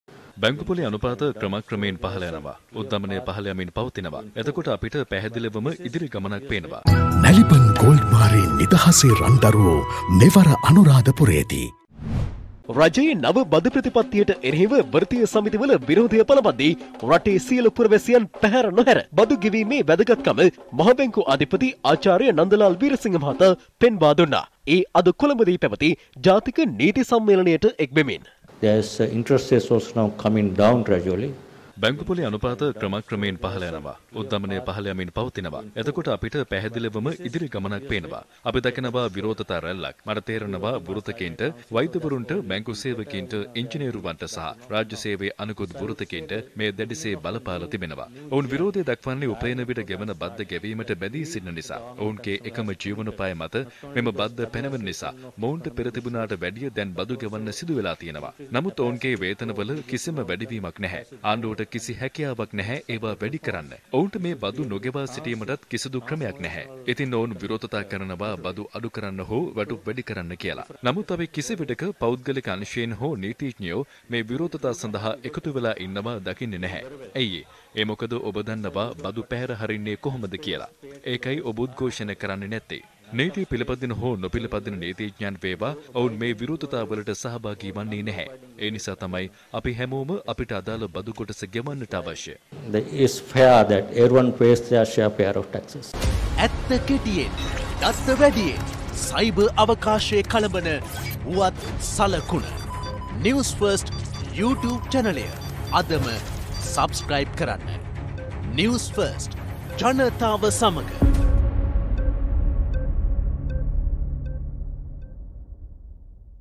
අපිට පැහැදිලිවම ඉදිරි ගමනක් පේනවා - මහ බැංකු අධිපති -
බැංකු පොලී අනුපාත පහළ යනවා. උද්දමනය පහළ යනවා